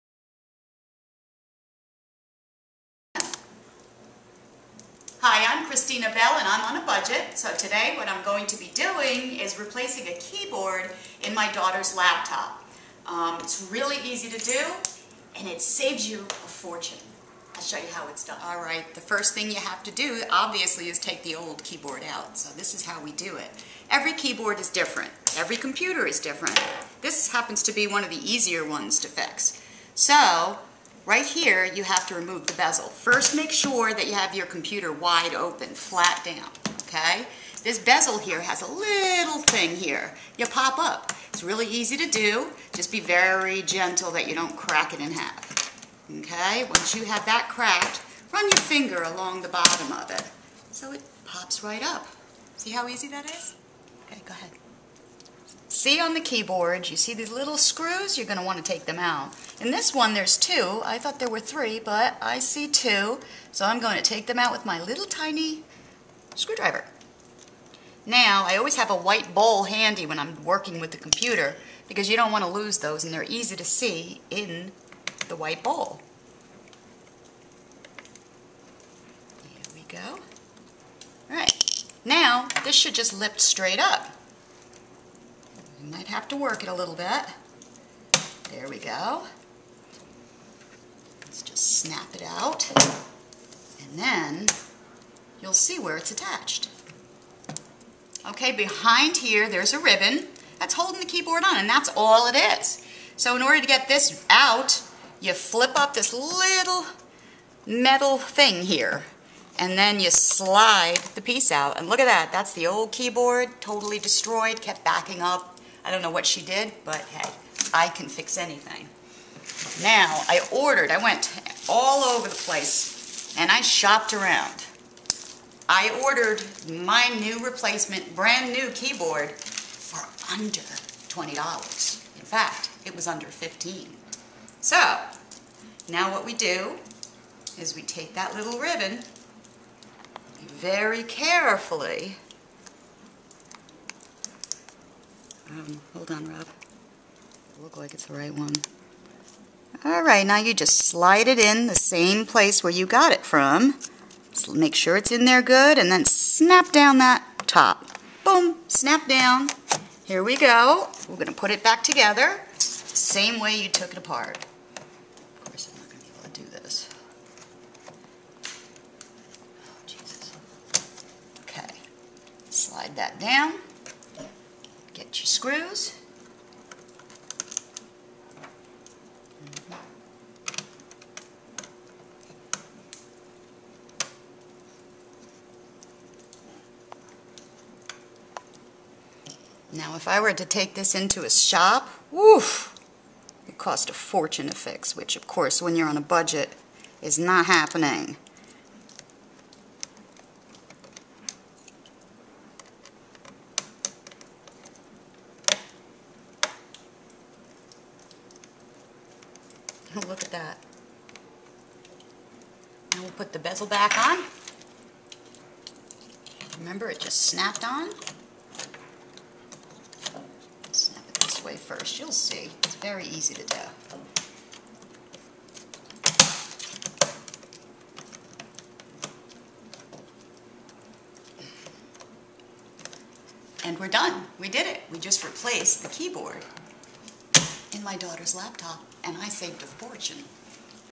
How to Replace a Dell Laptop Keyboard
Christina Bell replaces laptop keyboard on a budget, Dell Inspiron 1501 "easy laptop repair"